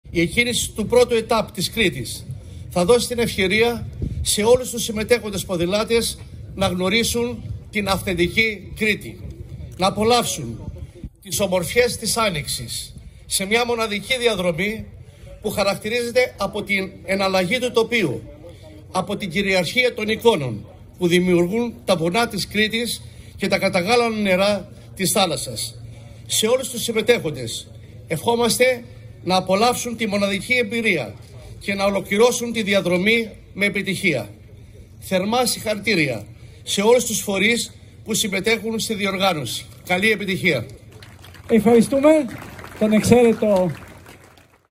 Στ. Αρναουτάκης ΔΕΗ tour of HELLAS
Ποδηλατικός γύρος

Η αναβίωση μιας σπουδαίας αθλητικής διοργάνωσης αποτελεί ένα γεγονός που μας γεμίζει αισιοδοξία. Ο ΔΕΗ Ποδηλατικός Γύρος Ελλάδας με τη συμμετοχή  σπουδαίων ομάδων και κορυφαίων αθλητών αντανακλά τη διεθνή φήμη του αγώνα και ταυτόχρονα αναδεικνύει την ταυτότητα της Ελλάδας, ως μιας χώρας που φιλοξενεί διεθνή αθλητικά γεγονότα με επιτυχία.

Παράλληλα, η εκκίνηση του 1ου ΕΤΑΠ της Κρήτης θα δώσει την ευκαιρία σε όλους τους συμμετέχοντες ποδηλάτες να γνωρίσουν την αυθεντική Κρήτη.

Να απολαύσουν τις ομορφιές της Άνοιξης σε μια μοναδική διαδρομή που χαρακτηρίζεται από την εναλλαγή του τοπίου, από την κυριαρχία των εικόνων που δημιουργούν τα βουνά της Κρήτης και τα καταγάλανα νερά της θάλασσας.

Σε όλους τους συμμετέχοντες ευχόμαστε να απολαύσουν τη μοναδική εμπειρία και να ολοκληρώσουν τη διαδρομή με επιτυχία.

Θερμά συγχαρητήρια σε όλους τους φορείς που συμμετέχουν στη διοργάνωση.